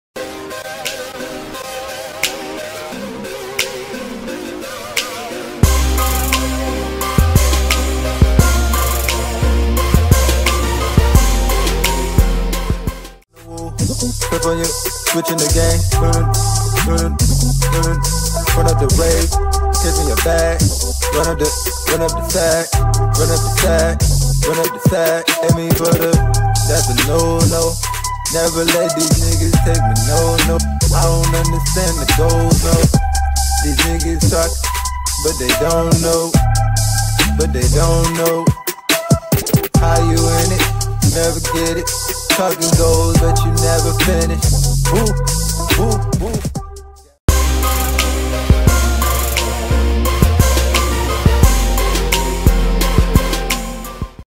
Poiz Performs at Direct 2 Exec Oakland 9/8/18 - Atlantic Records
Saturday, September 8, 2018 2:00 PM

Tooned Studios